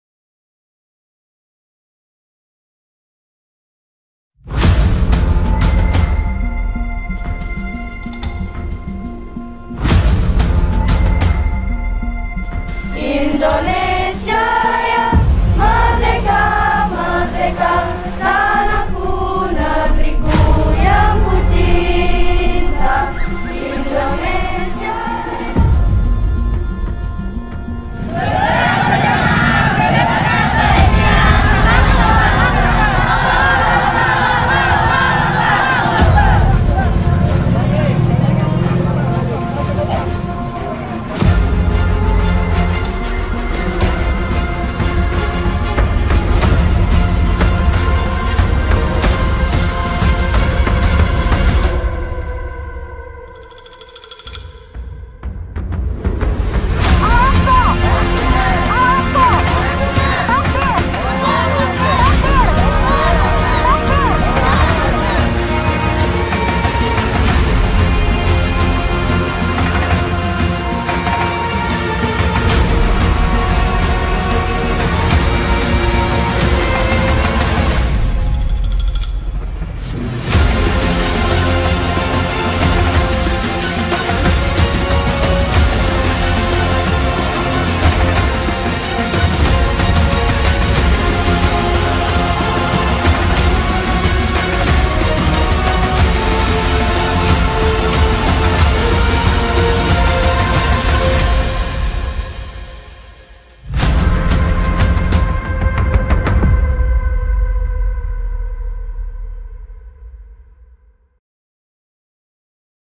SALAM CINTA DARI KALBAR - Trailer aksi damai peduli tahanan perempuan dan anak- anak Palestina
Trailer Dokumentasi Salam cinta dari kalbar